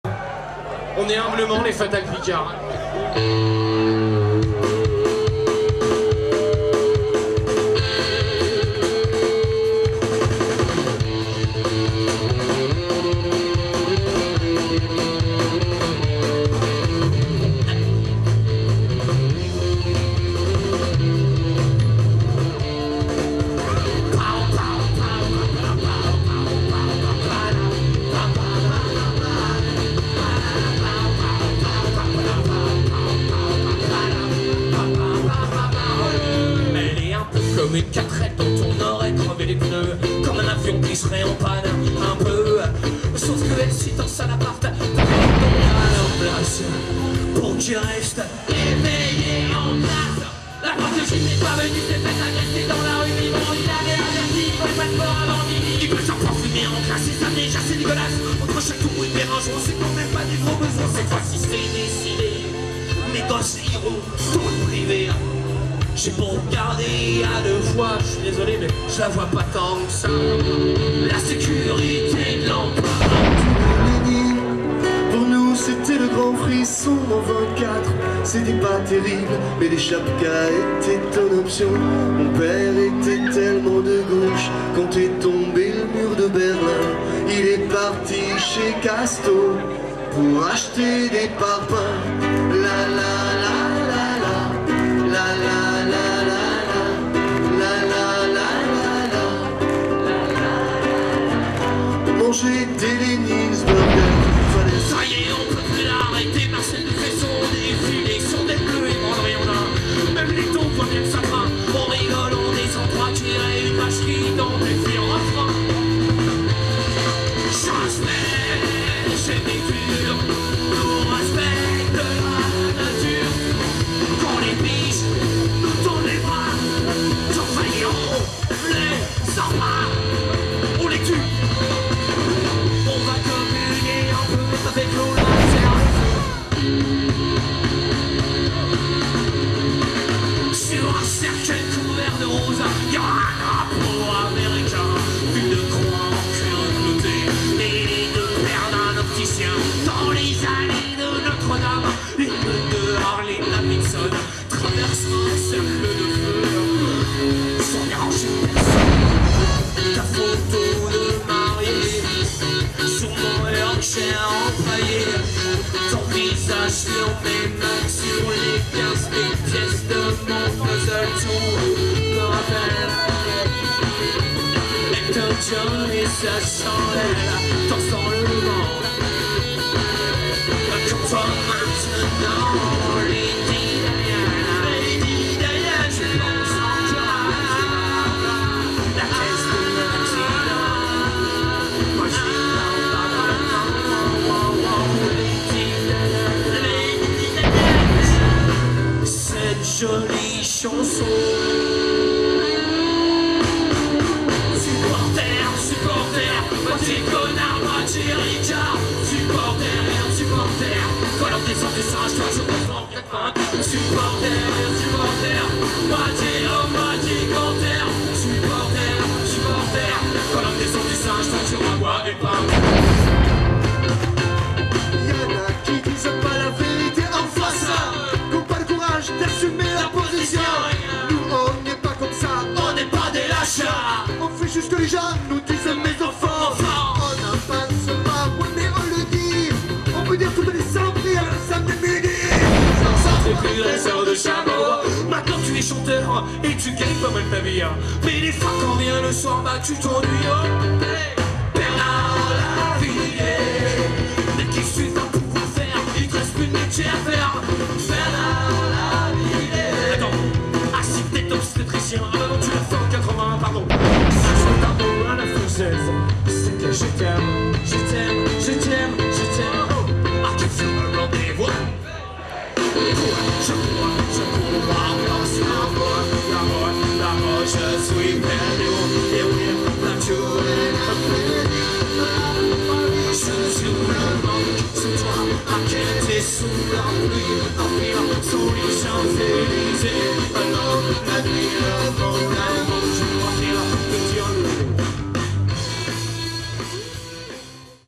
Les Fatals Picards - Festiv'Art Cirk'O'Zik 2009 - by Hamiral
Festival multiculturel de Cheniménil (88). - Programme 2009 : Les Fatals Picards, Ben Rain, la Place du Kif, Congopunq, Stratégie de Paix, Selekta Mao ...
Extraits du concert de chaque groupe également disponibles sur youtube